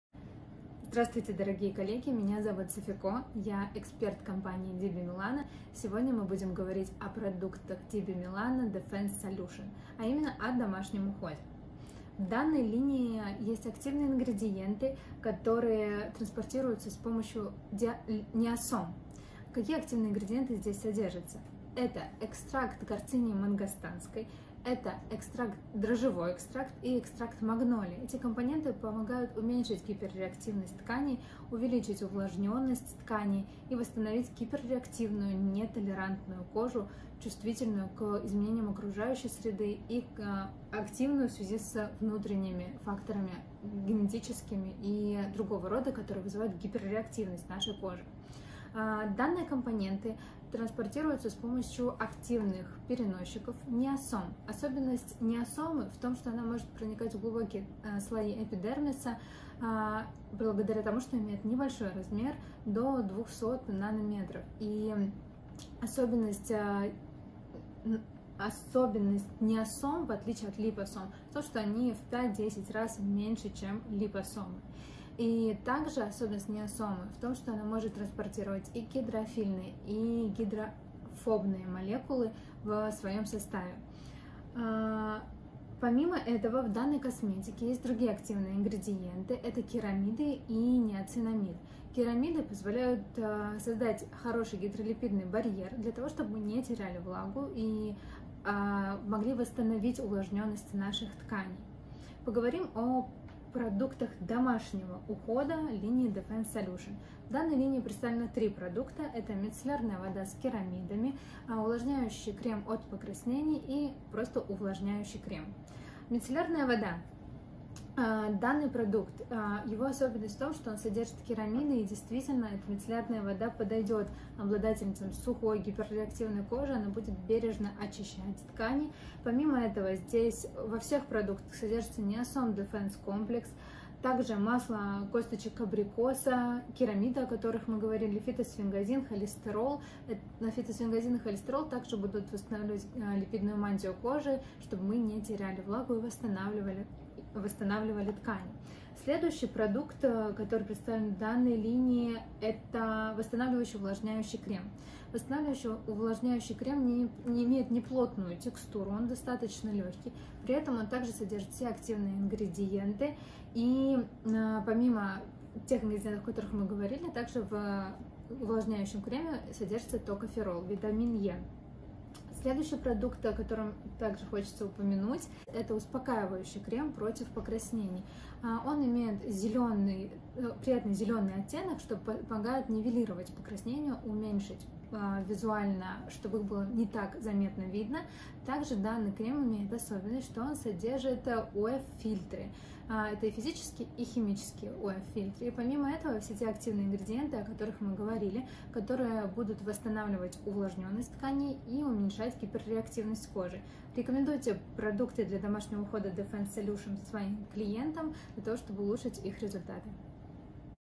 DIBI MILANO DEFENCE SOLUTION домашний уход
Тренер DIBI MILANO Софико Дараселия рассказывает о продуктах линии DIBI MILANO DEFENCE SOLUTION для домашнего ухода

ОПИСАНИЕ ЛИНИИ
Для чувствительной и нежной кожи характерны покраснение, зуд, реактивность и дискомфорт, ощущение жара и стянутости кожи. Эти проблемы могут появиться независимо от возраста, пола и типа кожи.

Чувствительная и нежная кожа гиперреактивна к внешним воздействиям, проявляет раздражение внезапно и часто — необъяснимо.

Спрос на косметику для чувствительной кожи* растет во всем мире. Возрастают и потенциальные источники сенсибилизации кожи: помимо генетических и гормональных (лекарства и физиологические изменения), появляются климатические, химические (загрязнение, дым), психологические (стрессы, недостаточное количество часов сна, эмоции) и поведенческие (длительное использование антиковидных масок).

DIBI MILANO отвечает на эти современные потребности созданием DEFENSE SOLUTION, специальной линии для чувствительной и реактивной кожи. Она действует против покраснений, повышенной чувствительности, сухости и зуда кожи. Чтобы успокоить, защитить и дать питание коже, сохранить баланс всех ее функций.